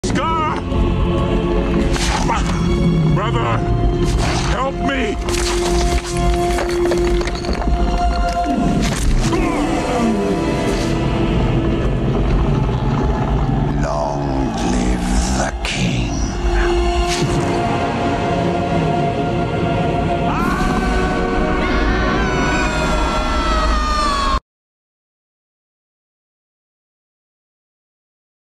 Me And My Friends On The Lion King Be Like Part 3
666 Twicefan - Scar
Jamerson.EXE - Mufasa
Aaron P. - Young Simba